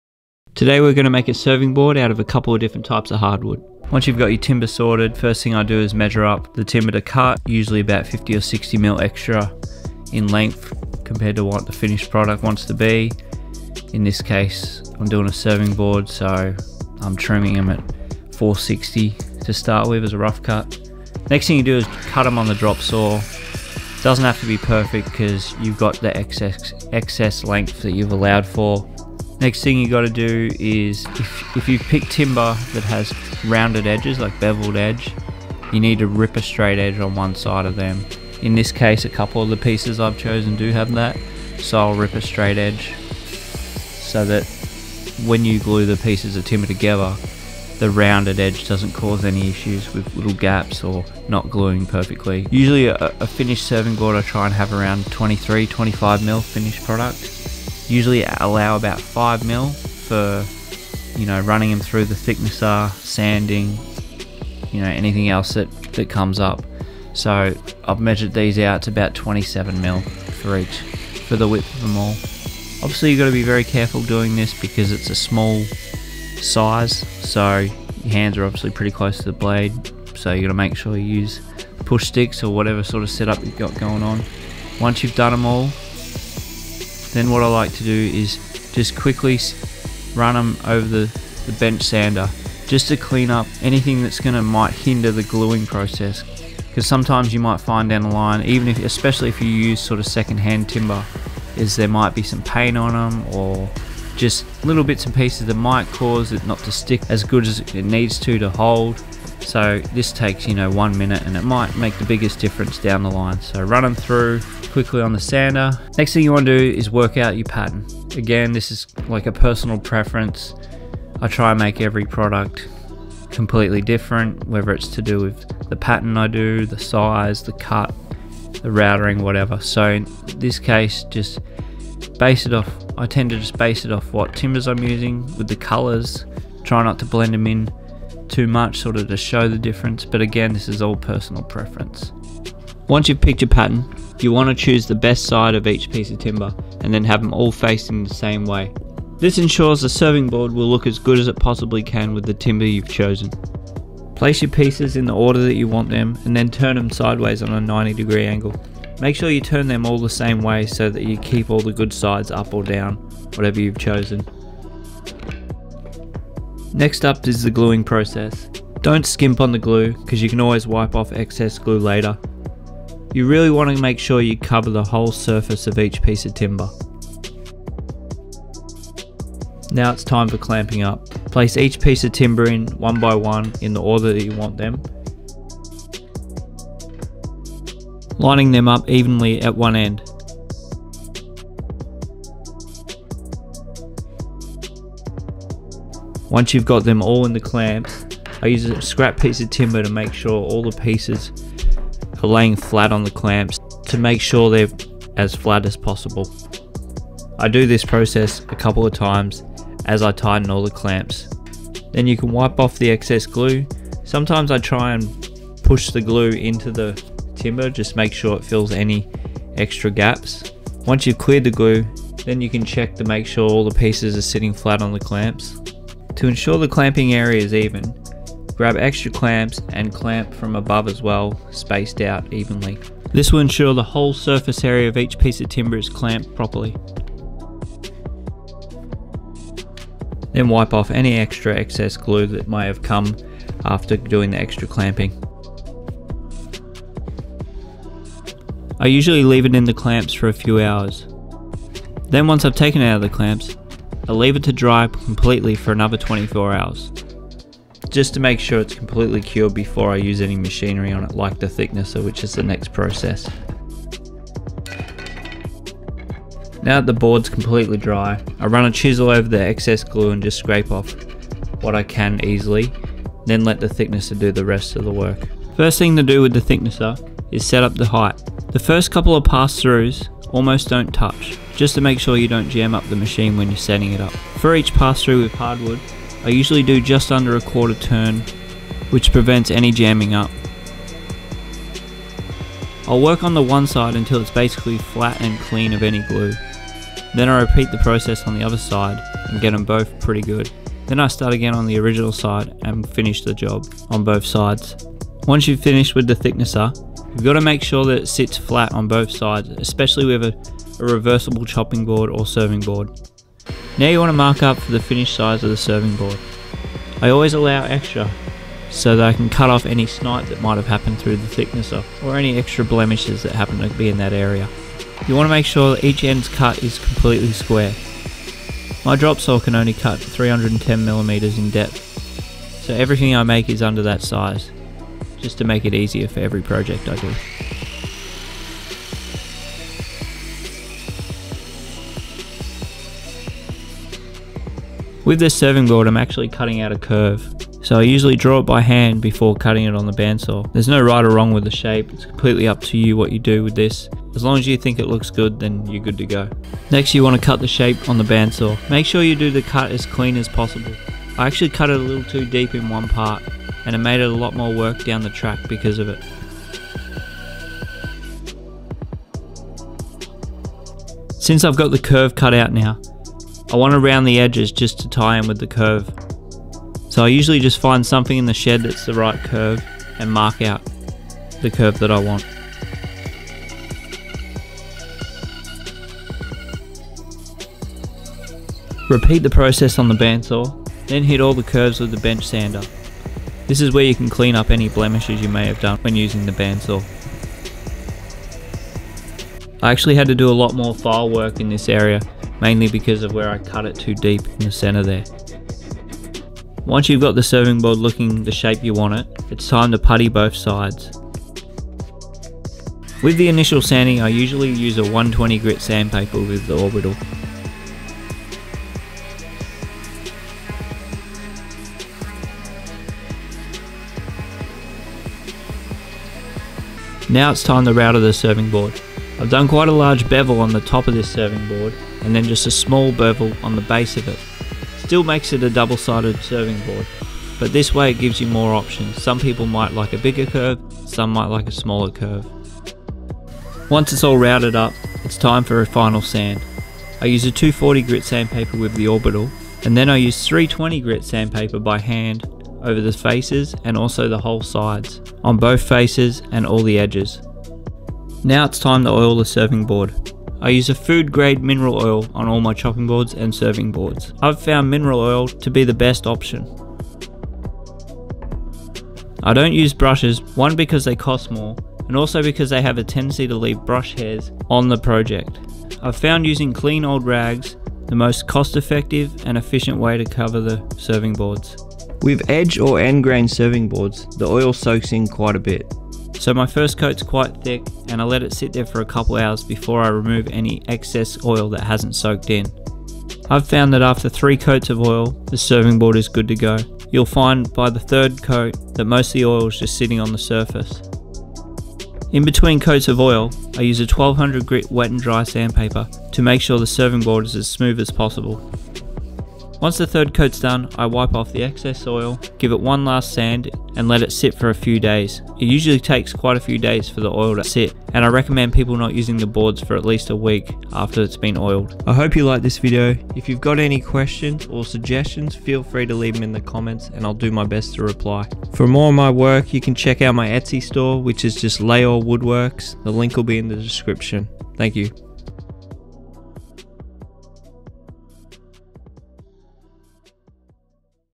How to make a Serving Board (Woodworking) / Layor Woodworks
In this video I show you how to make a serving board made out of multiple different types of hardwood timbers.

I am by no means a complete expert at this, it's just how I make them. Constructive criticism, questions or suggestions welcomed in the comments.